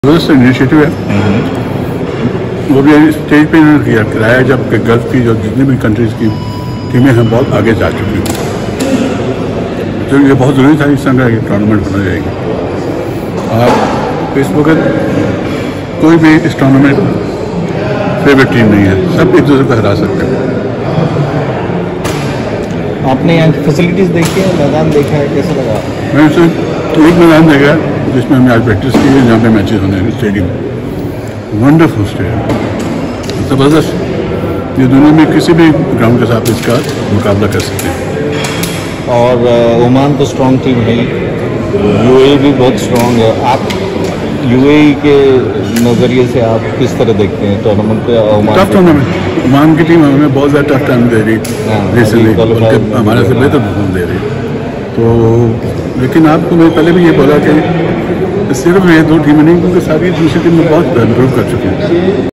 Former Pakistan opener Mudassar Nazar speaks on GCC T20 cricket in Doha
He is currently the consultant at UAE cricket association. UAE leads the six-team event. Tonight, they defeated Qatar.